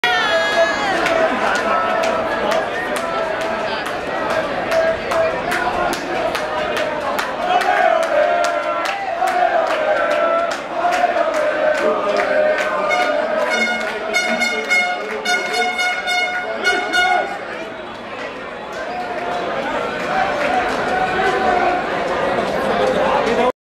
München, Marienplatz a BL-döntő előtt
Az Allianz Arénába és az Olympiastadionba tartó utasok az U3-as és az U6-os metró Marienplatz állomásán 2012. május 19-én.